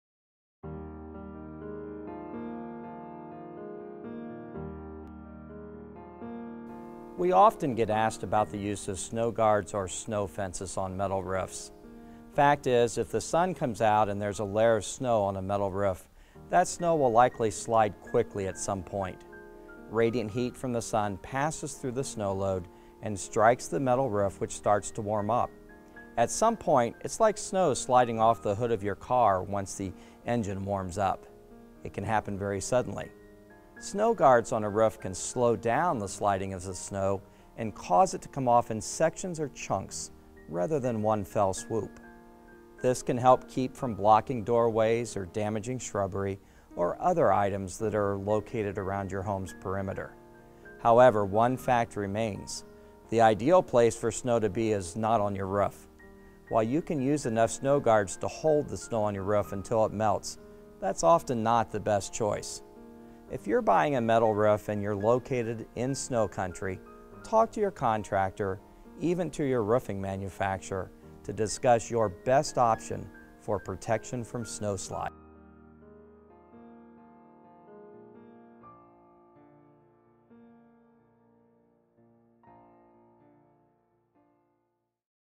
Snow Guards - Metal Roofing 101 with Todd Miller
We often get asked about the use of snow guards or snow fences on roofs. Fact is, if the sun comes out and there is a layer of snow on a metal roof, that snow will likely  slide quickly at some point. Radiant heat from the sun passes through the snow load and strikes the metal roof which starts to warm up. At some point, it is like snow sliding off the hood of your car once the engine warms up. It can happen very suddenly. Snow guards on a roof can slow down the sliding of the snow and cause it to come off in sections or chunks rather than one fell swoop. This can help keep from blocking doorways or damaging shrubbery or other items that are located around your home’s perimeter. However, one fact remains. The idea place for snow to be is not on your roof. While you can use enough snow guards to hold the snow on your roof until it melts, that is often not the best choice.  If you’re buying a metal roof and you’re located in snow country, talk to your contractor and even your roofing manufacturer to discuss your best option for protection from snow slides.